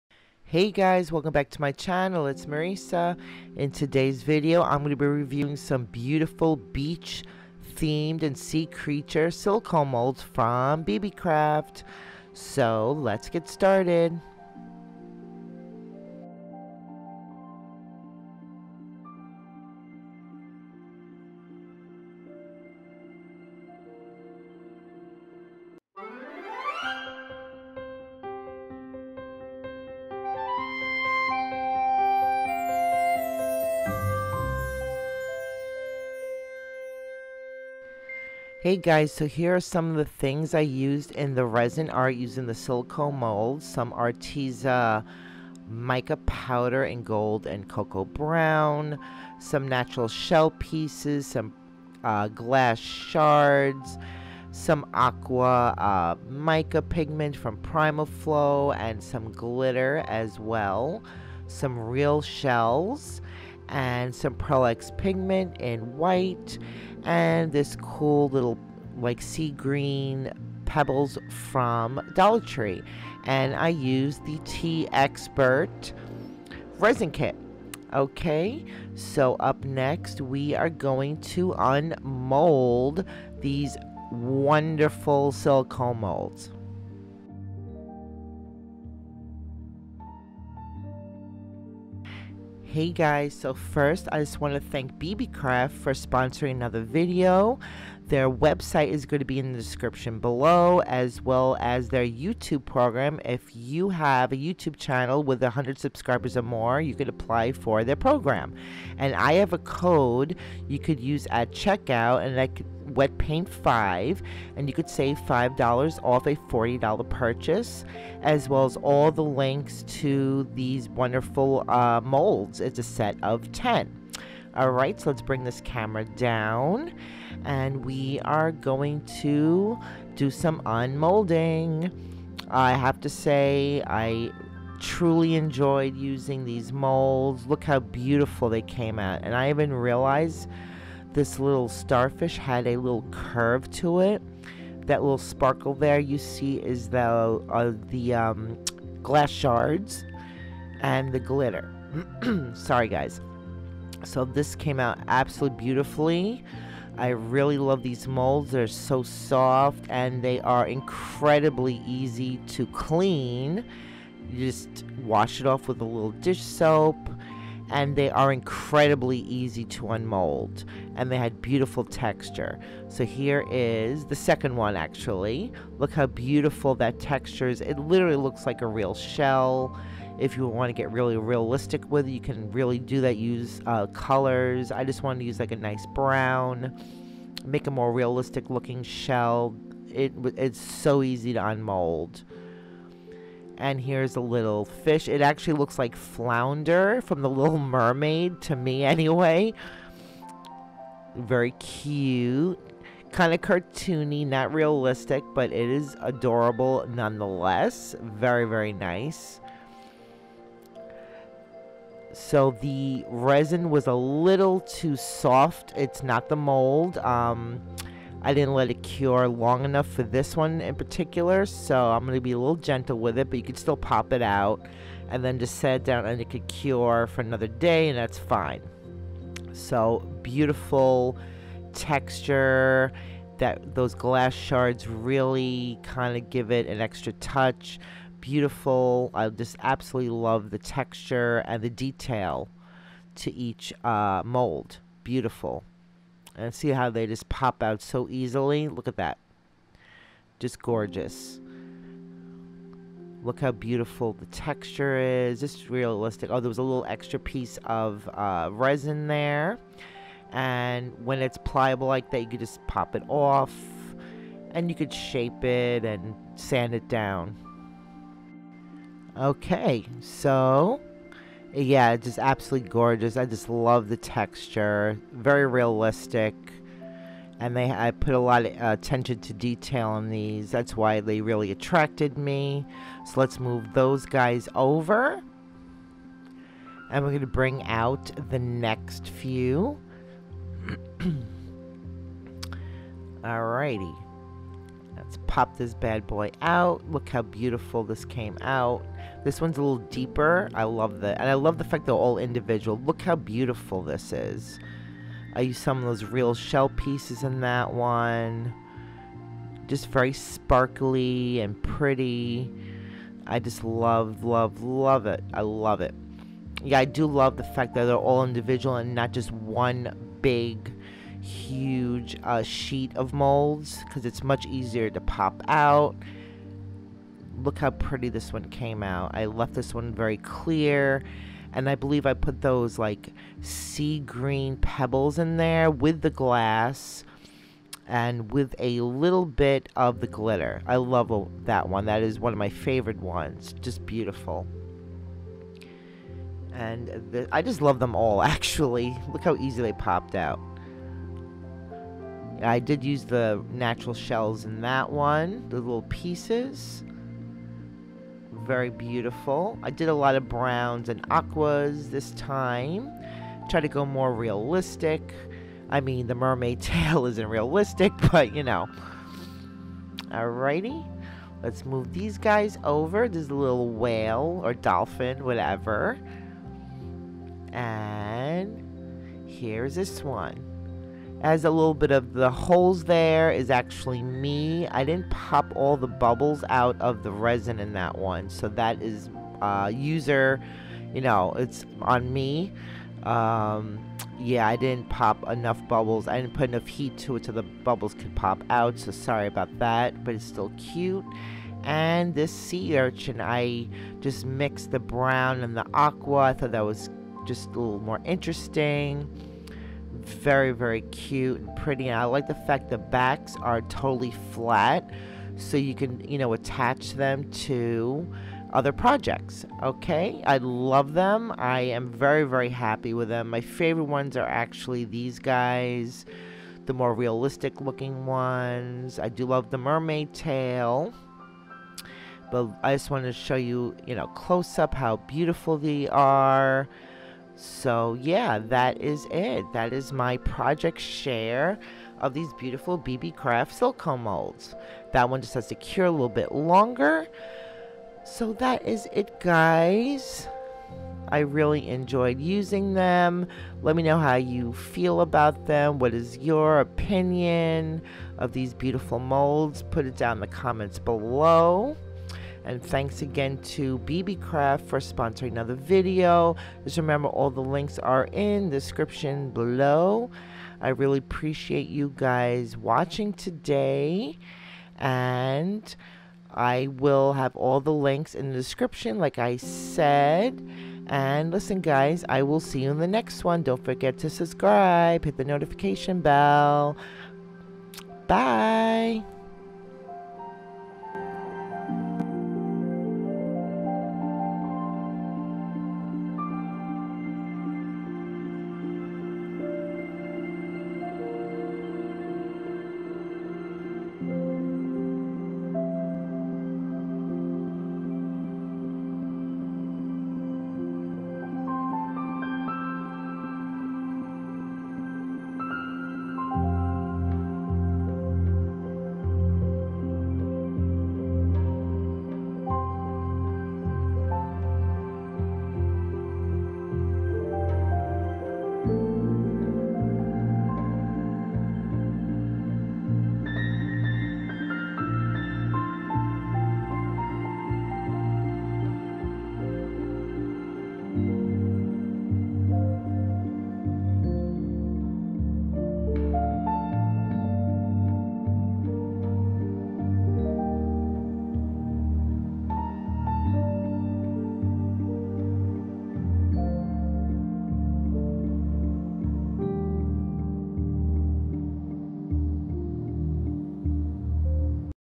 SPARKLY SEA CREATURES Resin Art & Beebeecraft Review
Hey guys, I hope you love these silicone molds by Beebeecraft as much as I do. They were fantastic to work with. Have a great day

Beebeecraft Youtube Program:
If you have a YouTube channel with 100 Subscribers or more, you can click the link and sign up.